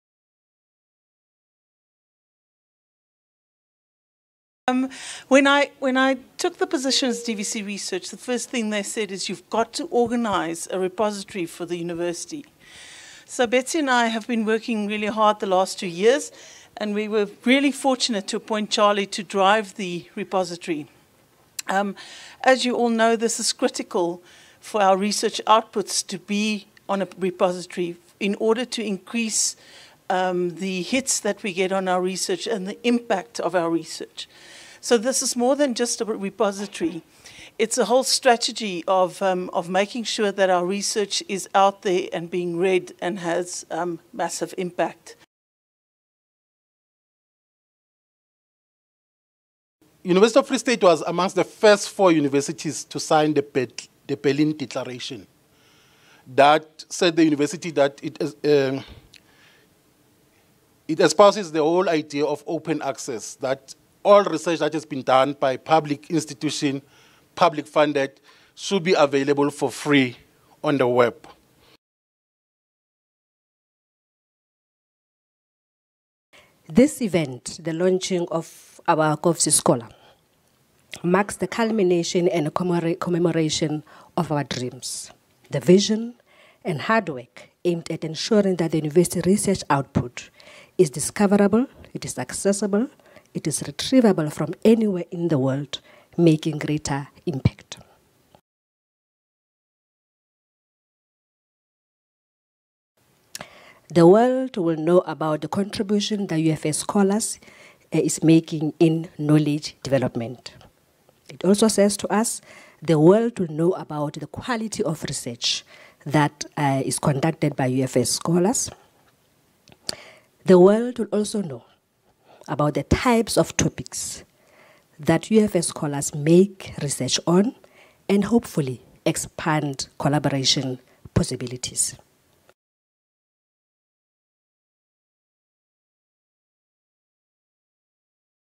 KovsieScholar launch 2015
From the KovsieScholar launch in 2015 with Vice-Rector: Research Prof Corli Witthuhn, Deputy Director: Scholarly Communications Mr Charlie Molepo, and Library Director Ms Betsy Eister.

KovsieSholar is a University of the Free State  digital service that collects, preserves, and distributes open access digital material. Repositories are important tools for preserving an organization's legacy; they facilitate digital preservation and scholarly communication.